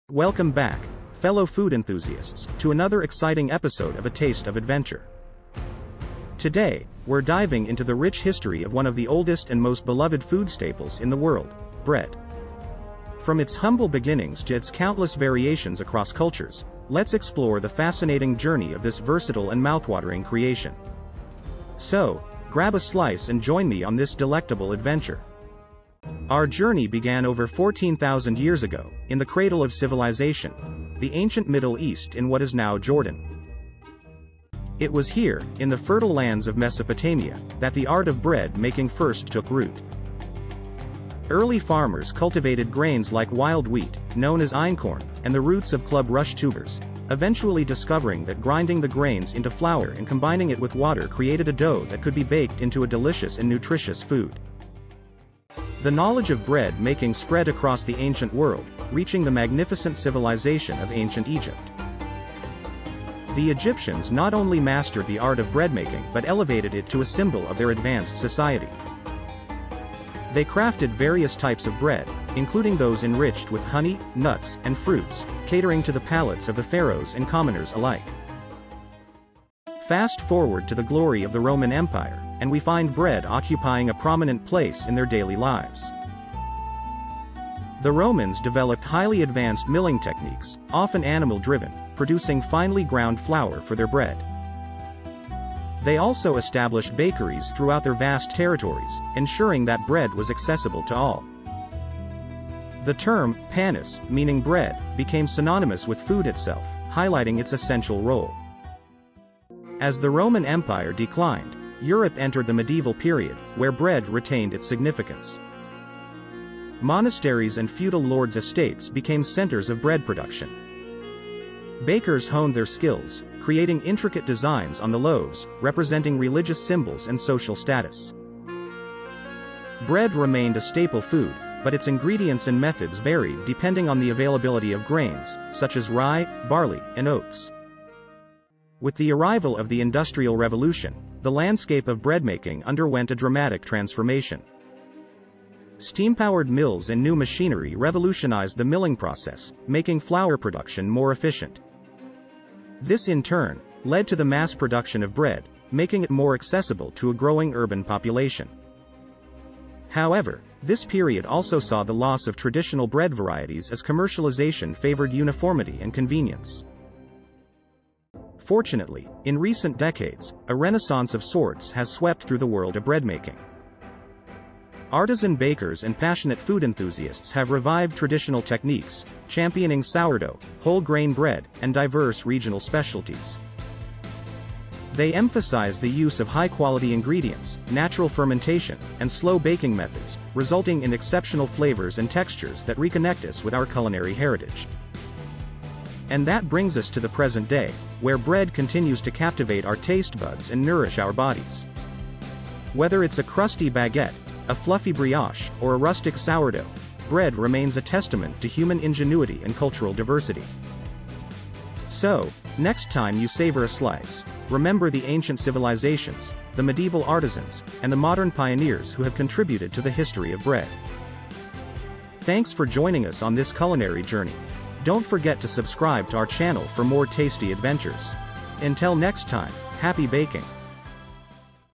From Ancient Grains to Modern Loaves: A History of Bread
Take a culinary journey through time and explore the fascinating history of bread! From ancient grains to modern loaves, discover how bakers around the world have been perfecting the art of bread-making for centuries. Get ready to satisfy your hunger for knowledge with our video: From Ancient Grains to Modern Loaves: A History of Bread